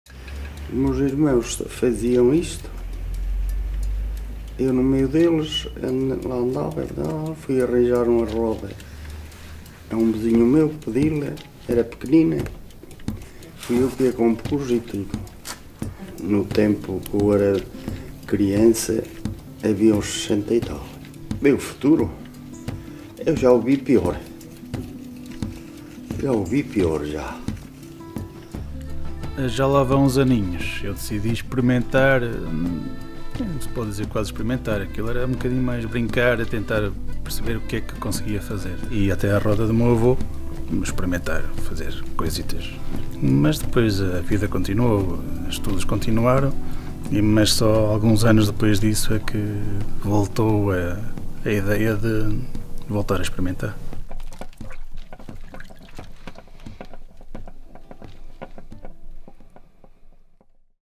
Bisalhães Black Pottery - The faces behind the tradition (Part I)
The years go by, but the Black Pottery of Bisalhães lives on!

Meet some of the faces who preserve this ancient tradition and who have contributed to it being considered Intangible Cultural Heritage by UNESCO.

Black Pottery of Bisalhães is considered Intangible Cultural Heritage by UNESCO, since 2016.